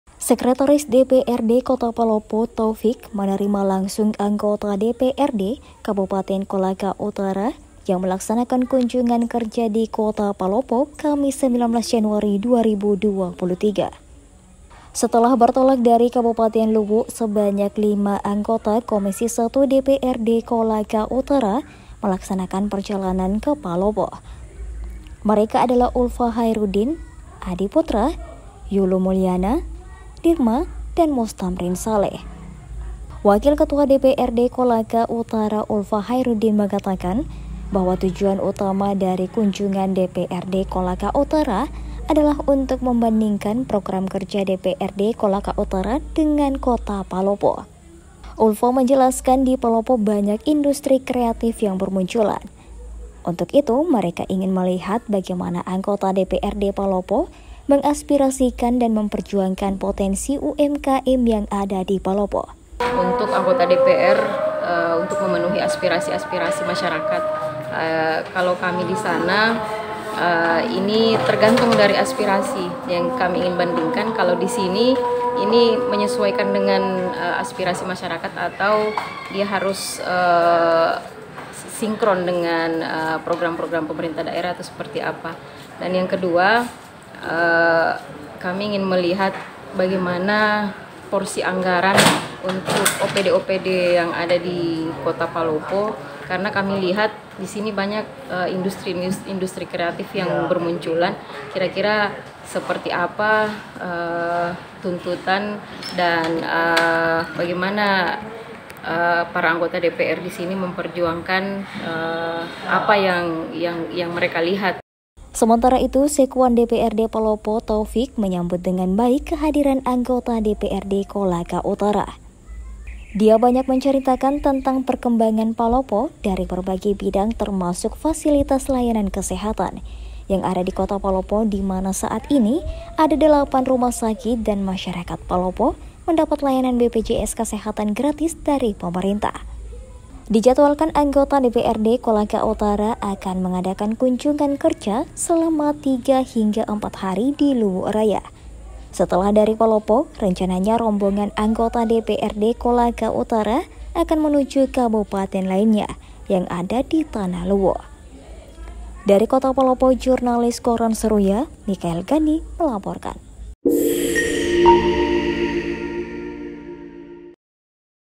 Kunjungan Kerja DPRD Kolaka Utara
Wakil Ketua DPRD Kolaka Utara : “Di Palopo Banyak Industri Kreatif”